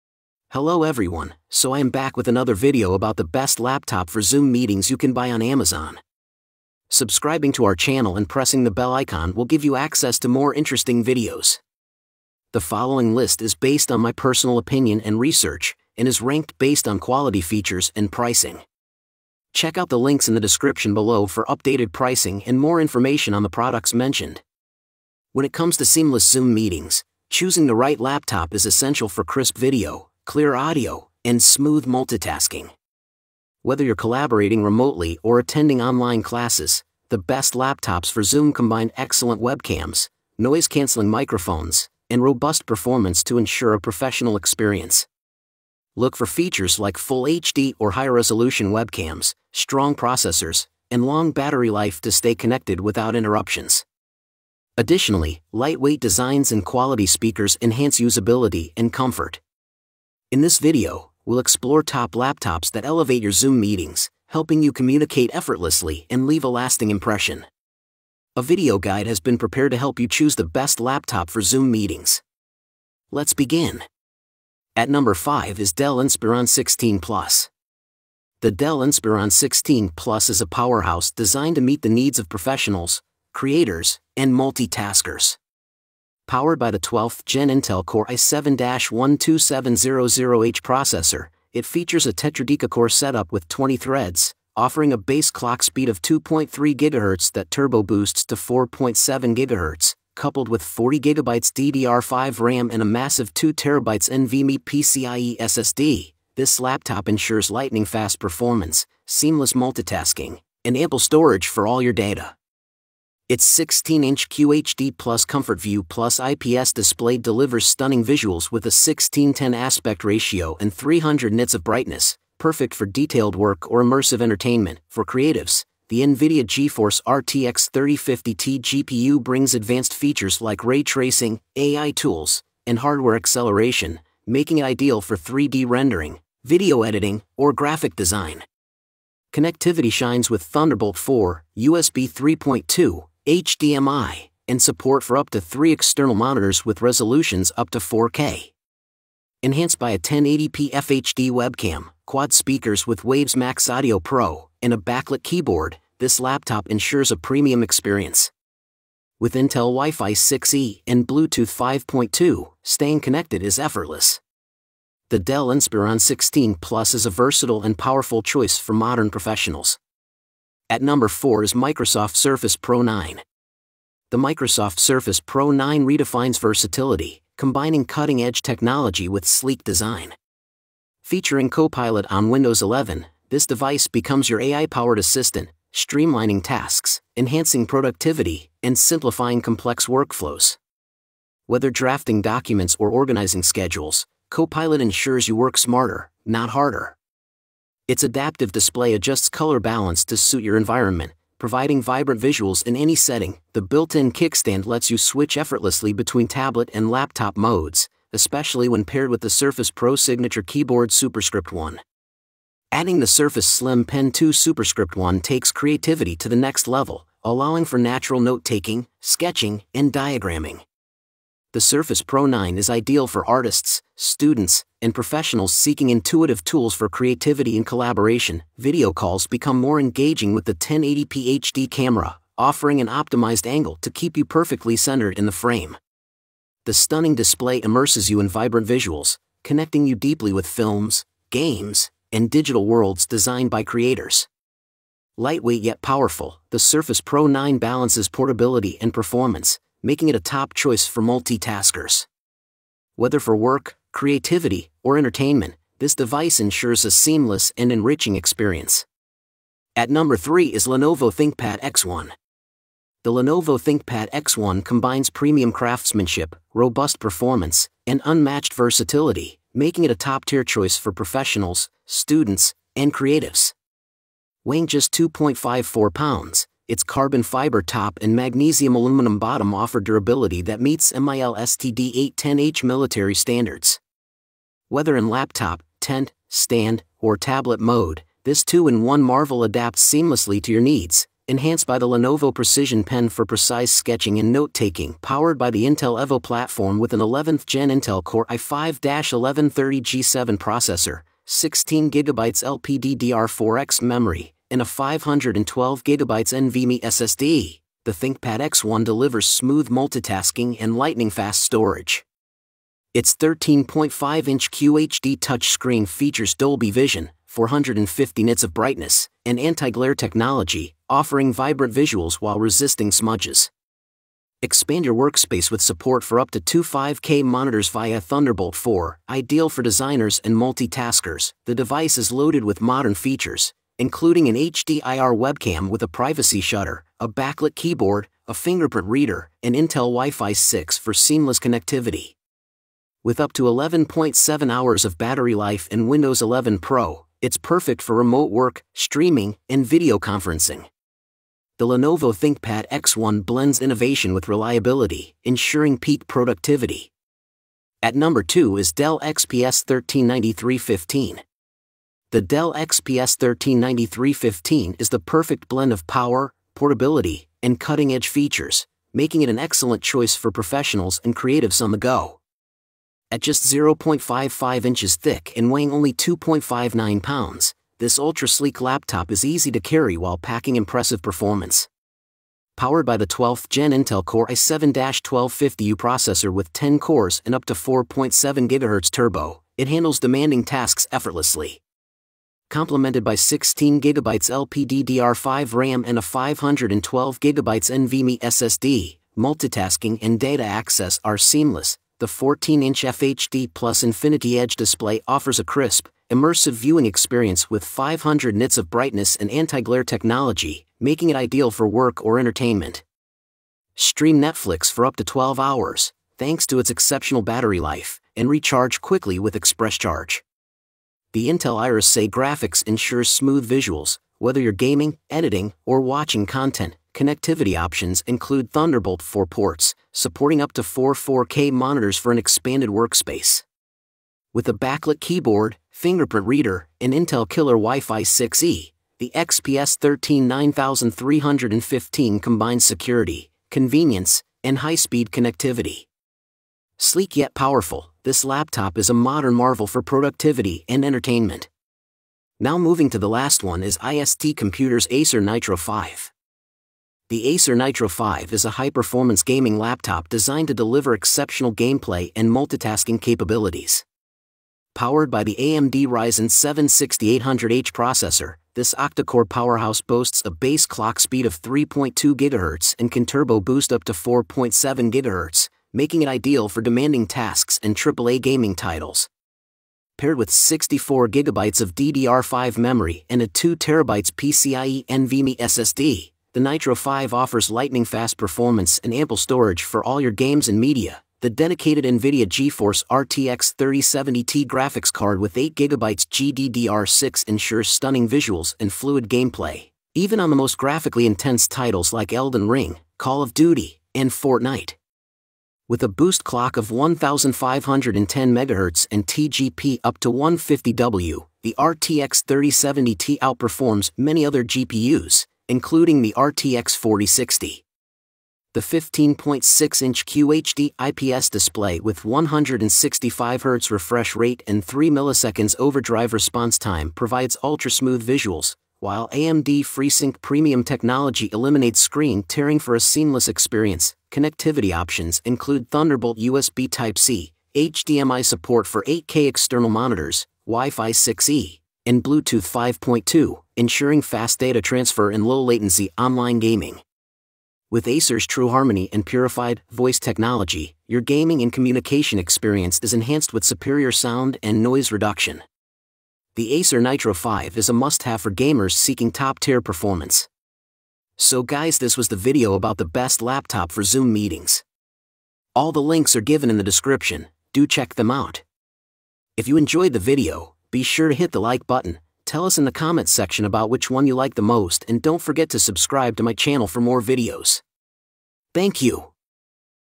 Top 5 Best Laptops for Zoom Meetings in 2025! (Fast, Clear & Reliable)
Looking for the best laptop for Zoom meetings in 2025? Whether you're working from home, attending online classes, or hosting virtual conferences, having a laptop with a high-quality webcam, crystal-clear audio, and smooth performance is essential. In this video, we’ll review the Top 5 Best Laptops for Zoom in 2025, covering features like Full HD webcams, noise-canceling microphones, powerful processors, and long battery life. Stay productive and professional with these top picks!

5 best laptop for zoom meetings:
introduction 0:00
5:Dell Inspiron 16 Plus 1:15
4:Microsoft Surface Pro 9 2:51
3:Lenovo ThinkPad X1 4:20
2:Dell XPS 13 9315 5:58
1:IST computers Acer Nitro 5 7:33

💻 Best Laptops for Video Calls & Remote Work
✔️ HD Webcam & Noise Cancellation
✔️ Long Battery Life & Fast Performance
✔️ Ideal for Zoom, Skype, Microsoft Teams & Google Meet